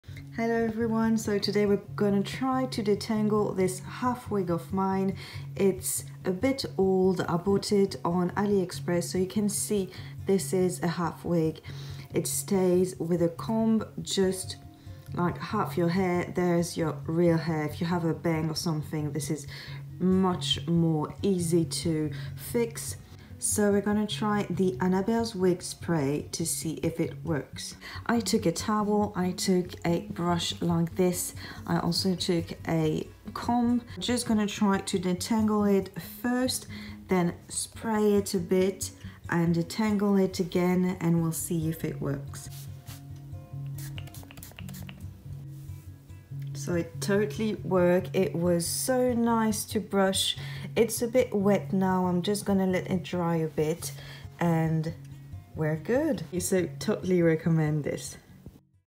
ANNABELLES WIGS LEAVE ON CONDITIONER SPRAY FOR SYNTHETIC HAIR WIG
Trying this leave on conditioner from Annabelles wigs , lets see !

As always if you want me to add any infos, if you have any requests please let me know.